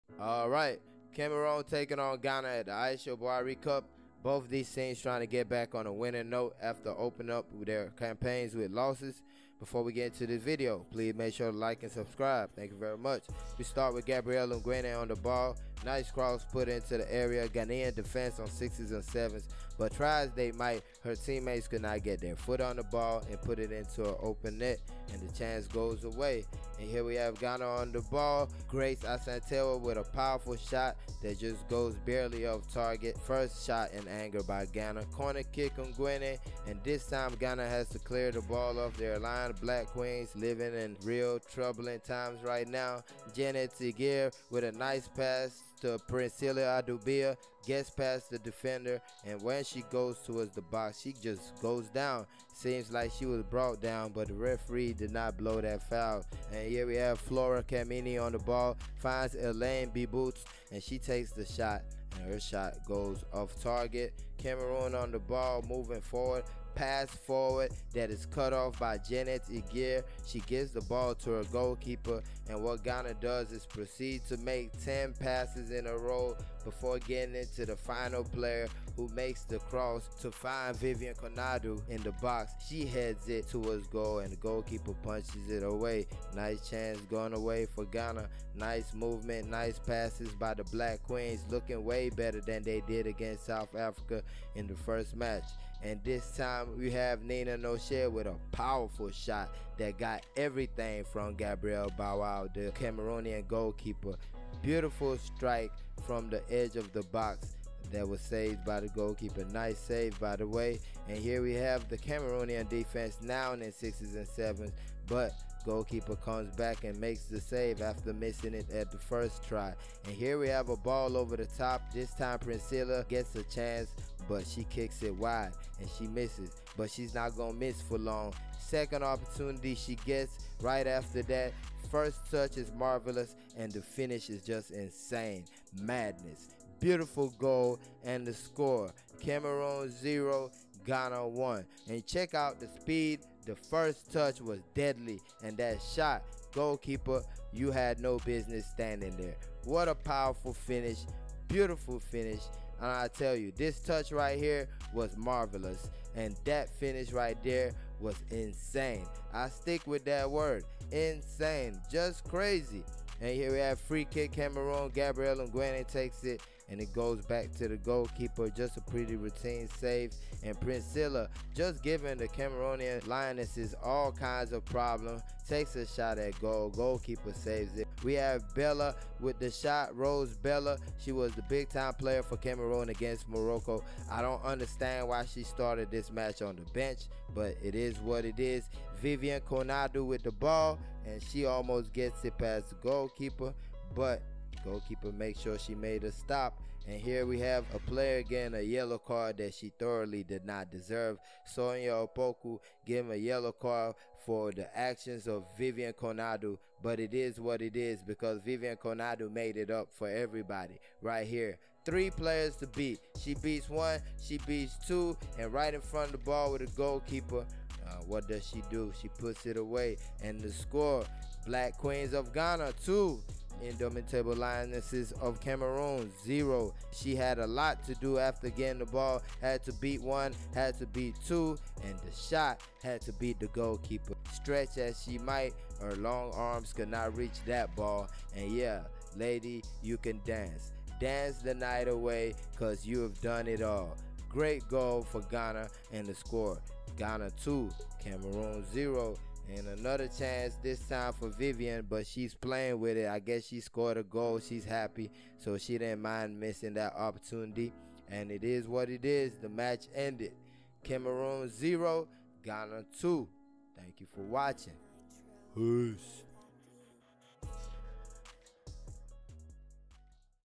Ghana vs Cameroon [2-0] Aisha Buhari Cup 2021 I Women's International Friendly
The Black Queens of Ghana ended their ABC campaign with a two goal victory over Cameroon.